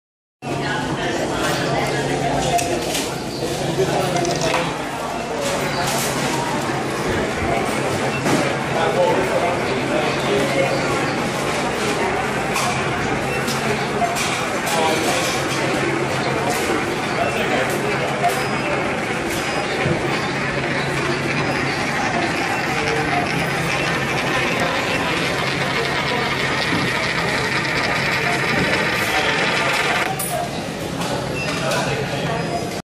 Coin Donation Vortex
Seeing one of these when you're a kid makes any shopping trip ten times better.
I'm not a kid anymore, but couldn't resist the temptation.